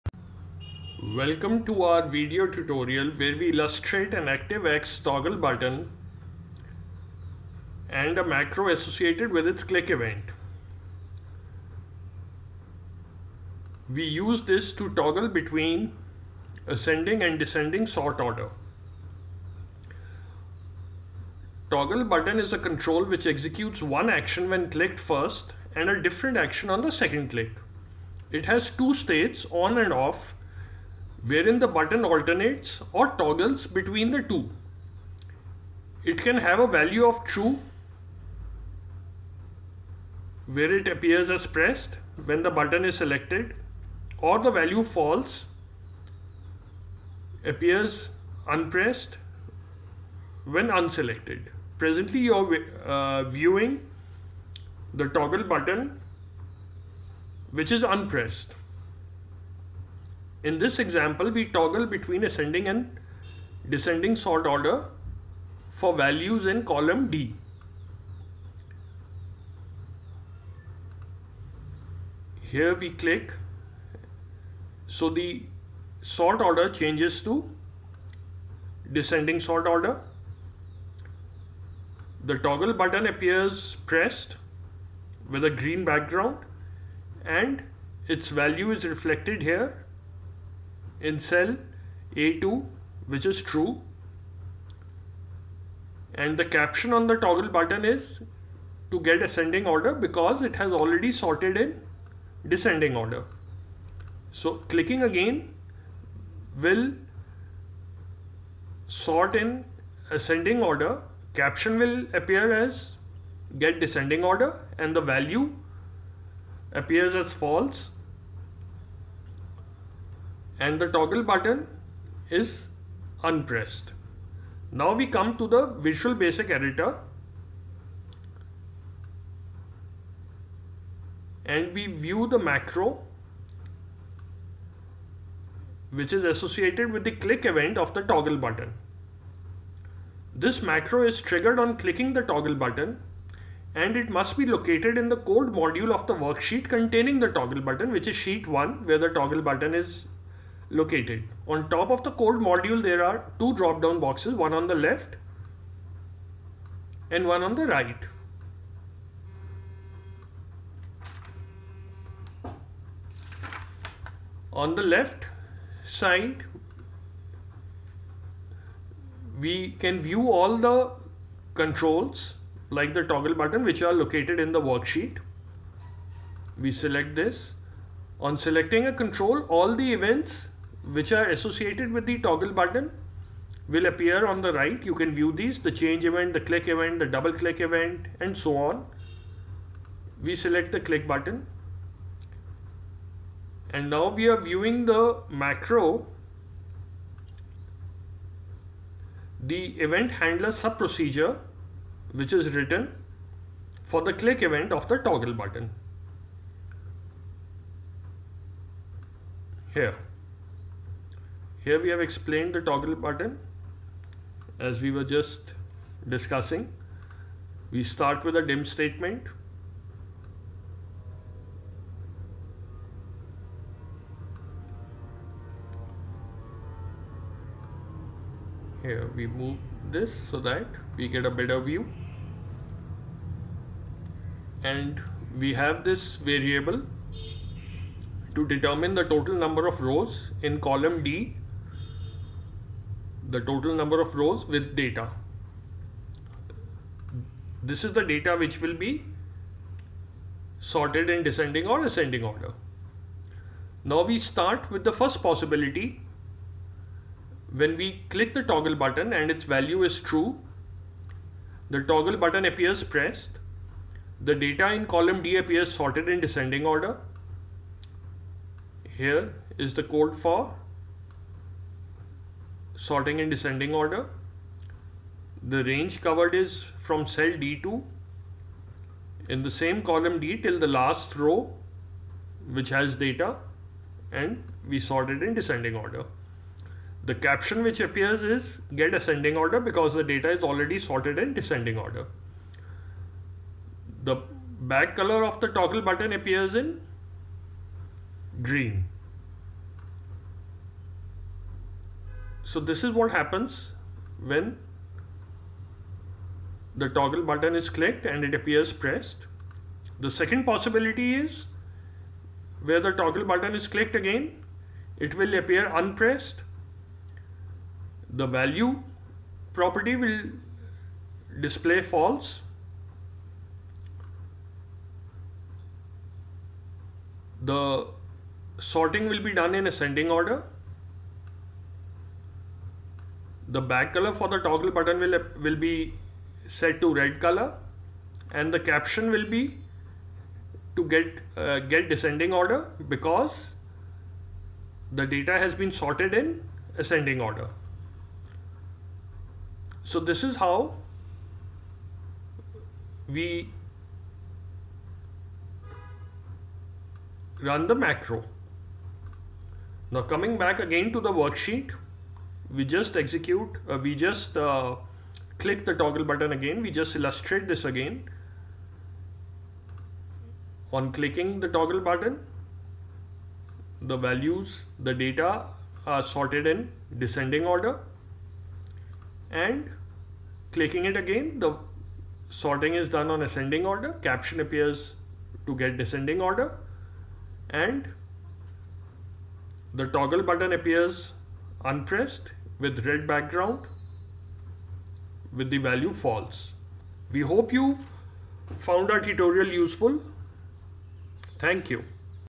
ActiveX Toggle Button Event Procedure in Excel VBA
Illustrate using an ActiveX ToggleButton in a Worksheet & Macro associated with its Click event, in Excel VBA, to toggle between Ascending and Descending Sort order.